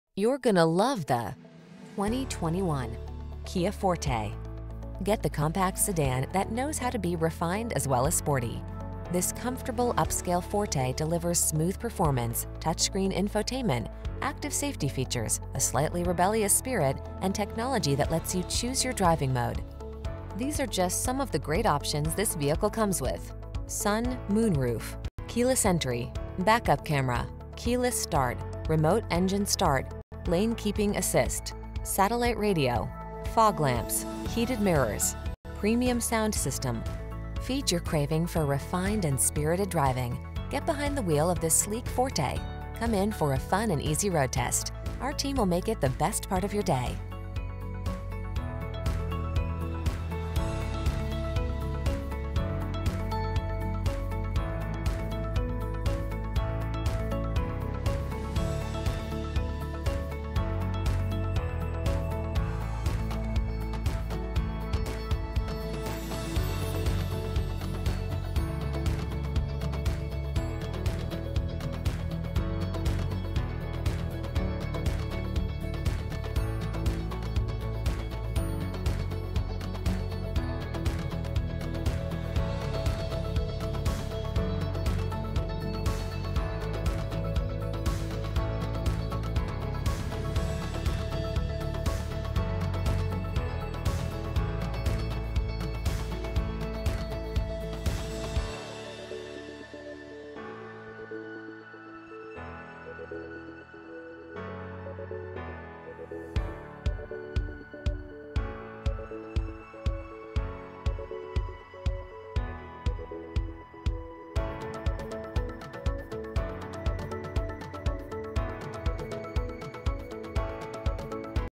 2021 Kia Forte Albany, Schenectady, Colonie, Troy, Glens Falls, NY K21F1238
Currant Red New 2021 Kia Forte available in Albany, New York at Destination Kia. Servicing the Schenectady, Colonie, Troy, Glens Falls, NY area.

Destination Kia
760 Central Ave